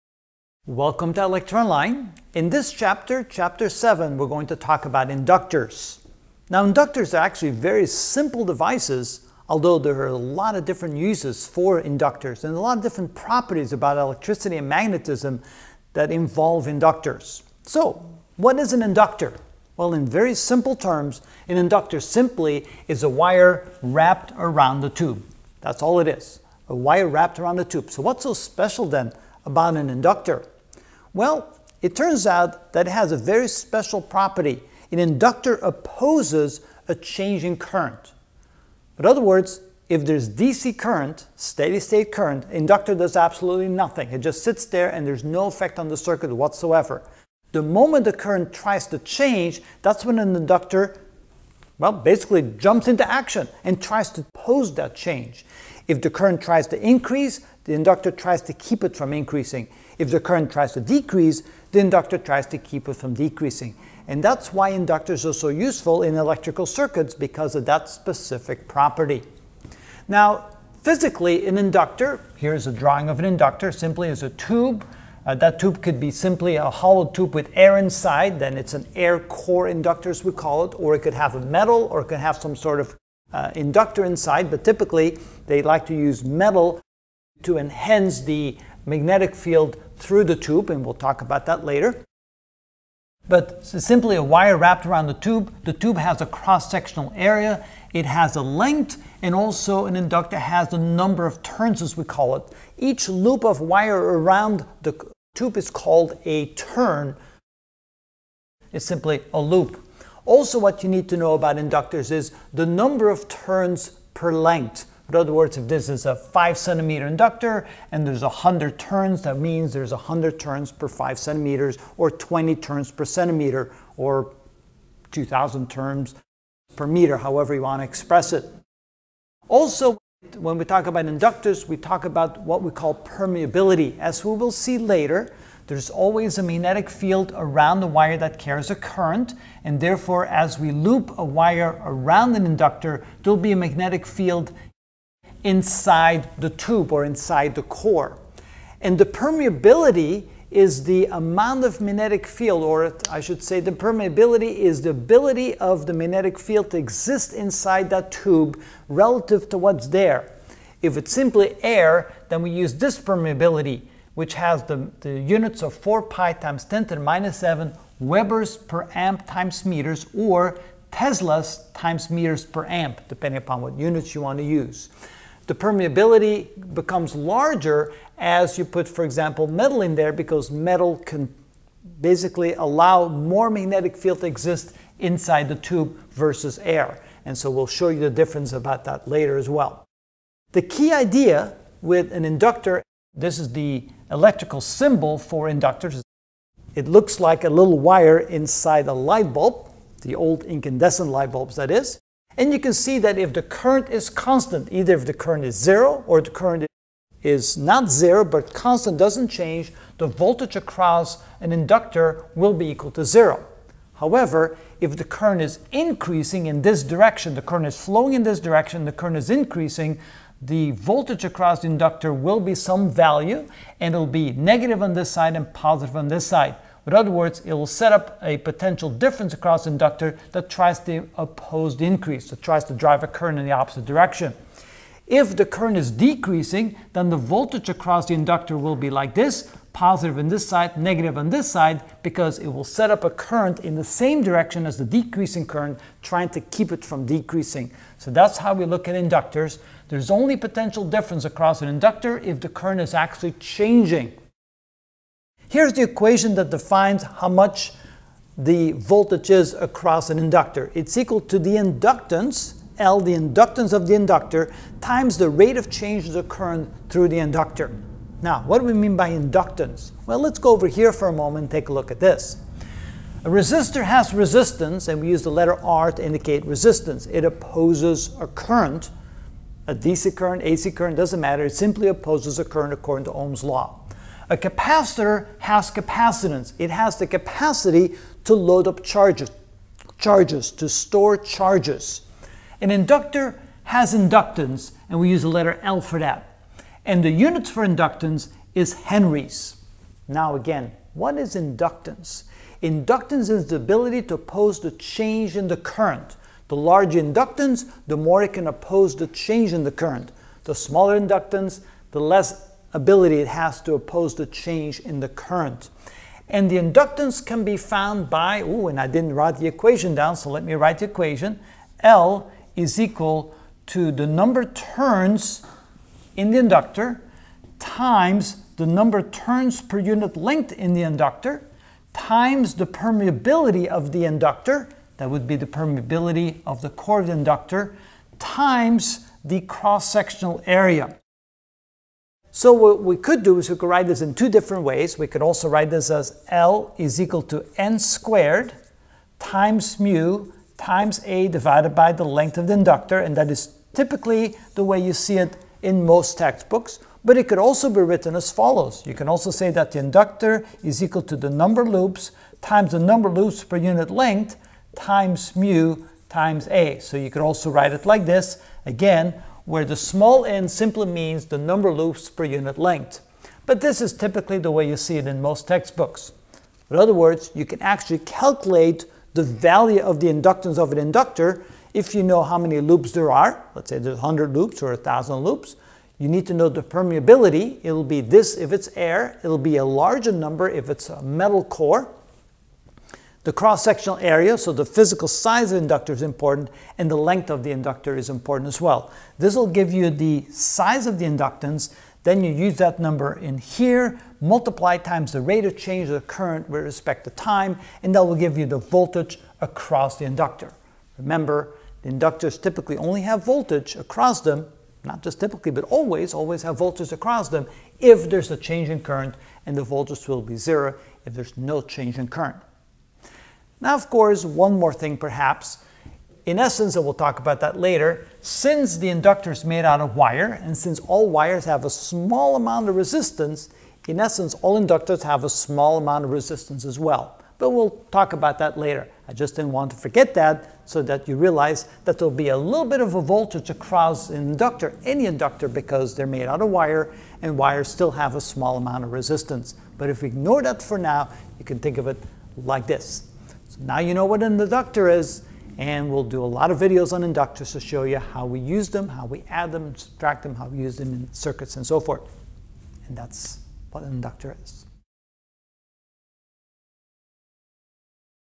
Electrical Engineering: Ch 7: Inductors (1 of 20) What is an Inductor?
In this video I will explain what is an inductor.